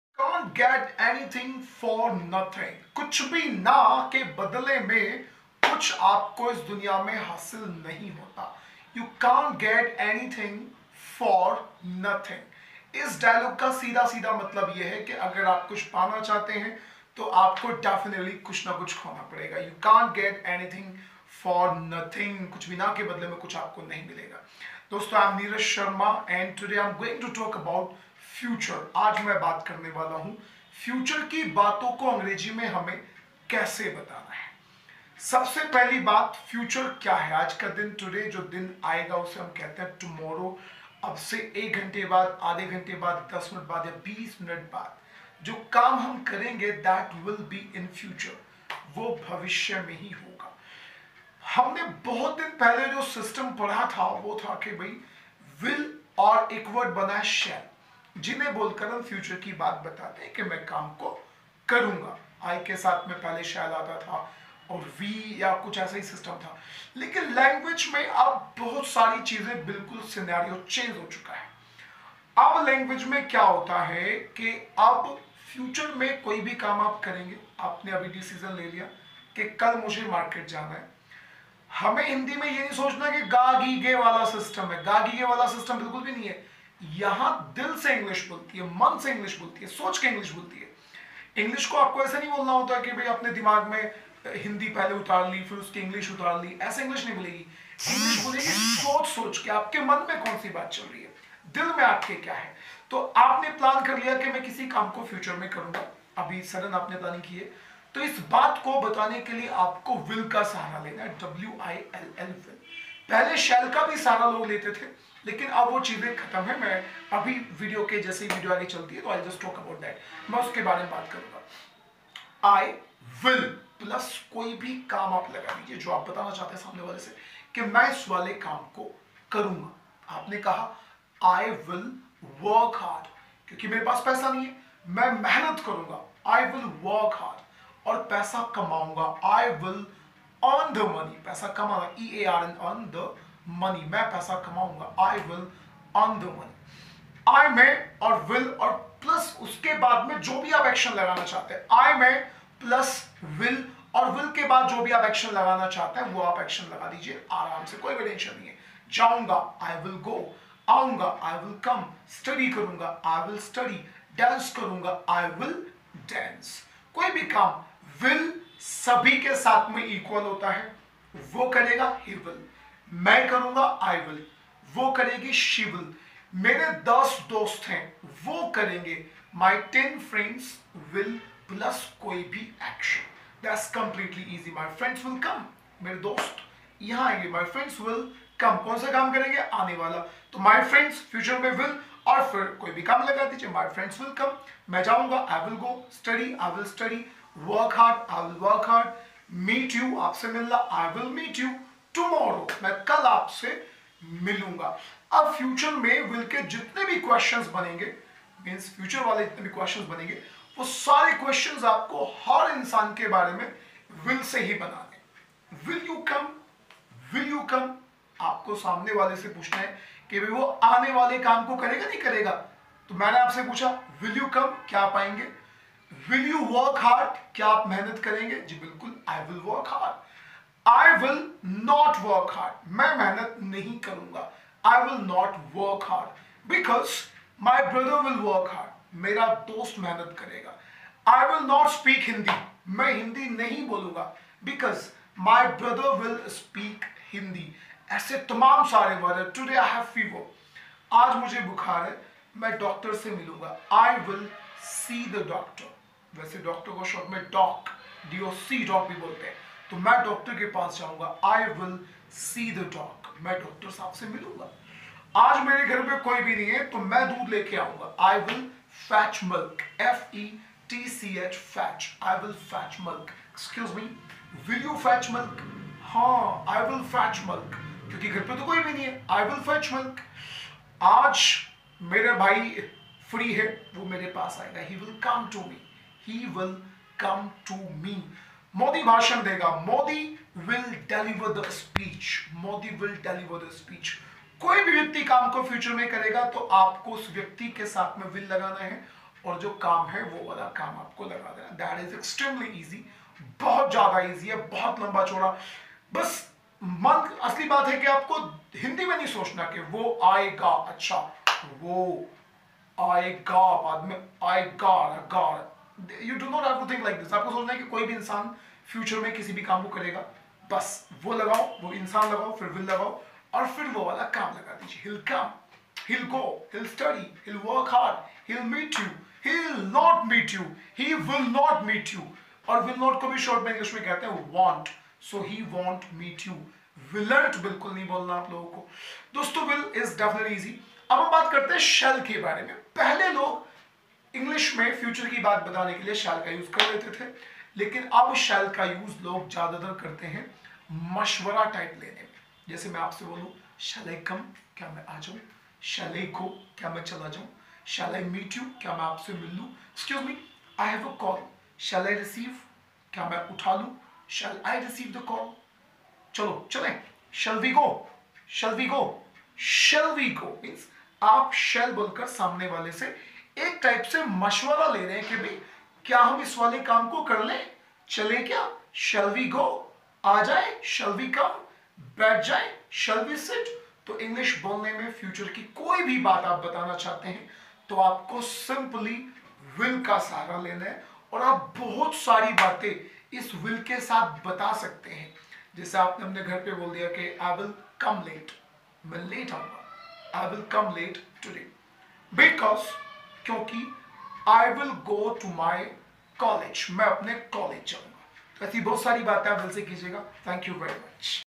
use of will and shall in a very easy way
I am trying my level best to define will and shall with a very easy way for English speakers so that things turn easier for them to use these two words.by these students can easily speak grammar of future..so plz like share and subscribe the channel for regular updates